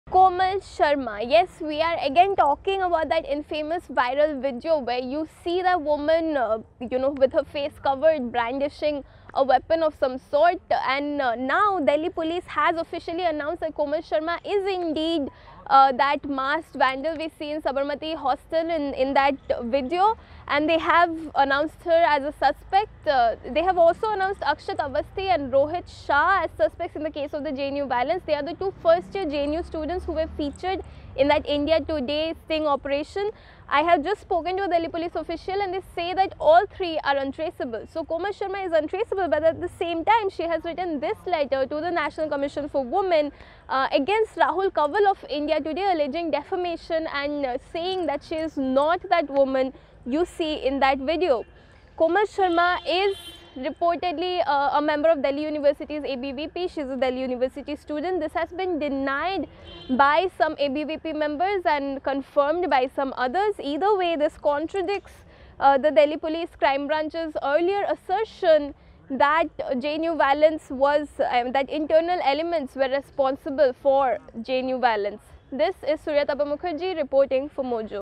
Police Suspect in JNU Violence, Komal Sharma is Absconding but Wrote a Letter to NCW on Monday
Komal Sharma, the masked woman in the viral video of JNU violence, is now a police suspect. But the police is unable to trace her. She as well as Akshat Awasthi and Rohit Shah have not shown up despite police summons. But just this Monday, Sharma had written a letter to NCW alleging defamation against Rahul Kanwal of India Today.